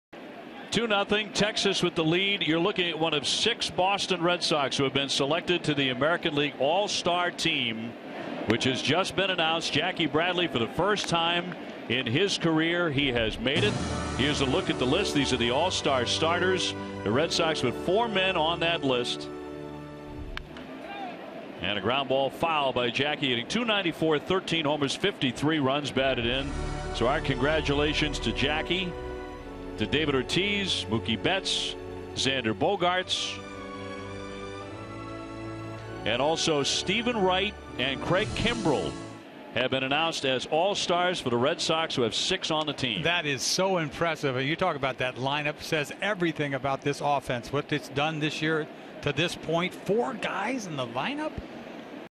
TEX@BOS: Broadcast on Red Sox's All-Star selections
The Red Sox's broadcast talks about Boston's AL-leading four All-Star starters, plus Steven Wright and Craig Kimbrel's selections